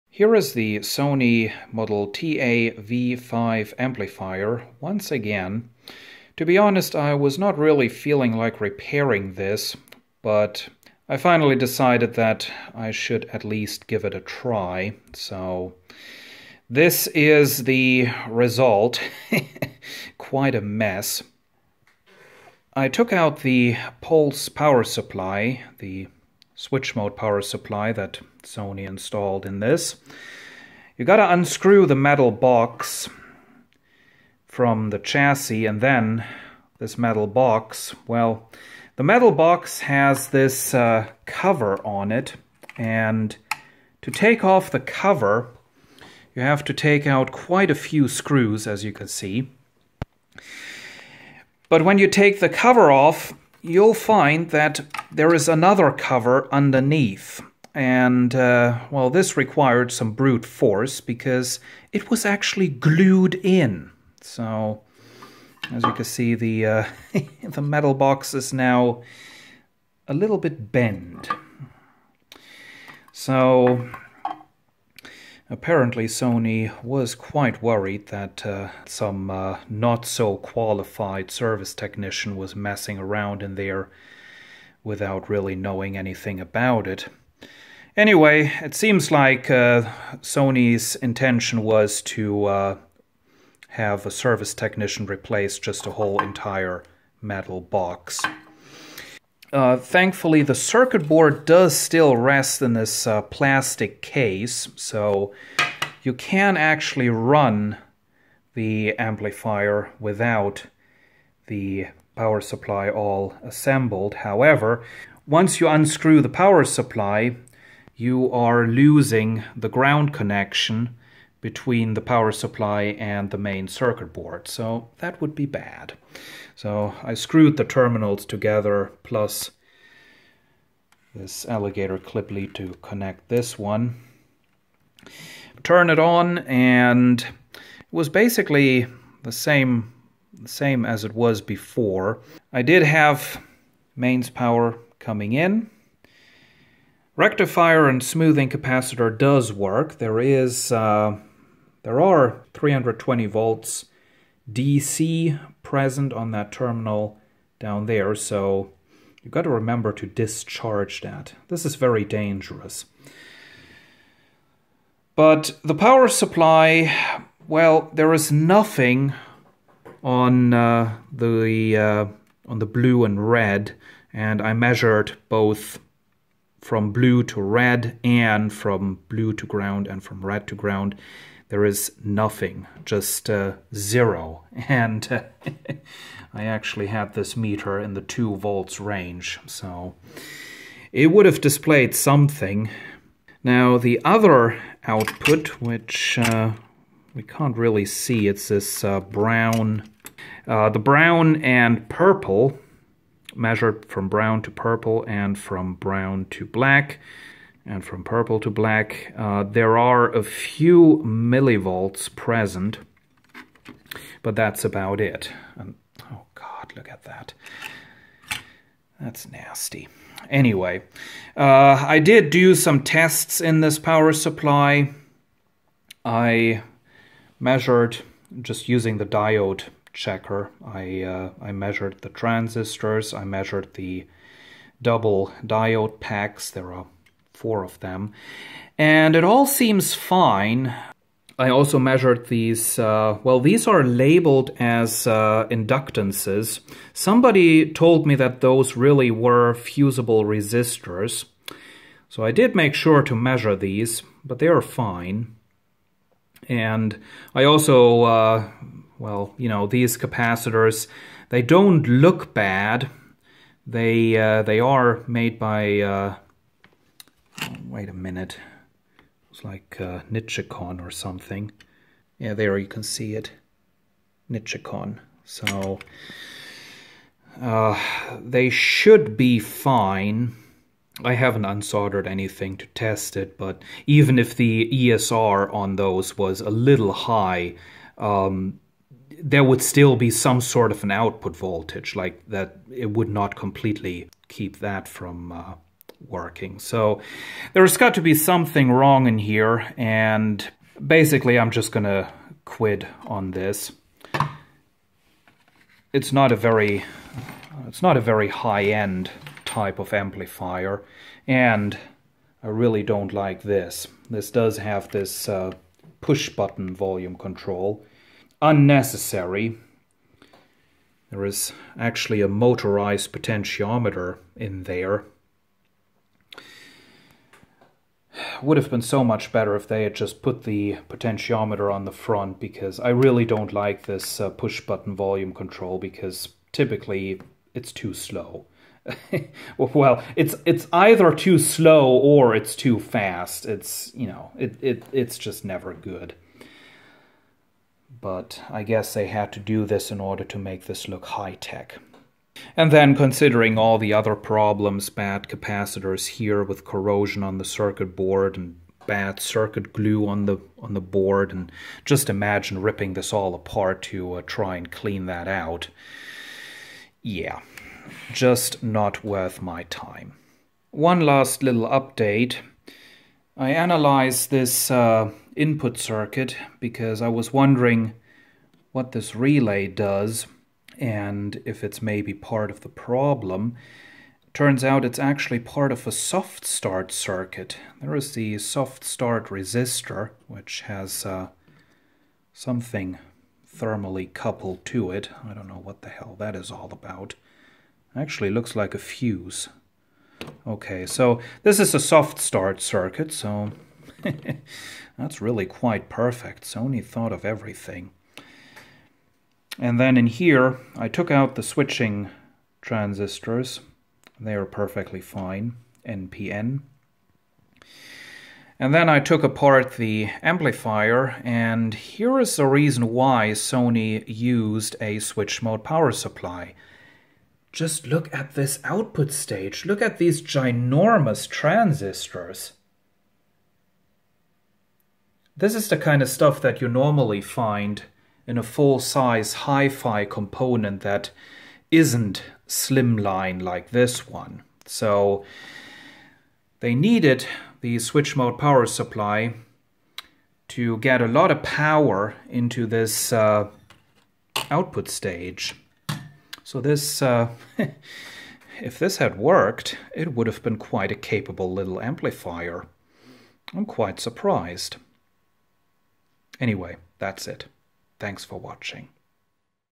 A closer look at the Pulse Power Supply of the Sony TA-V5 amplifier
I took the switched-mode power supply of the Sony TA-V5 amplifier apart to see if it could be fixed. Here is what I found.